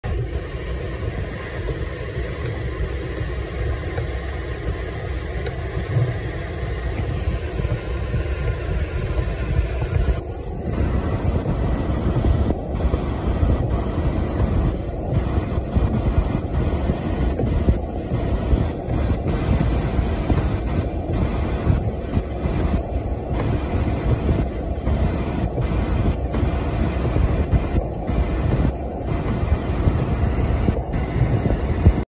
Snow plow has to wait for duck to make up its mind
During the recent April Noreaster in Maine, this driver who was plowing their driveway had to yield to this duck who didn't seem to be particularly bothered by being in the way.
Credit: Jami Murphy via Chime In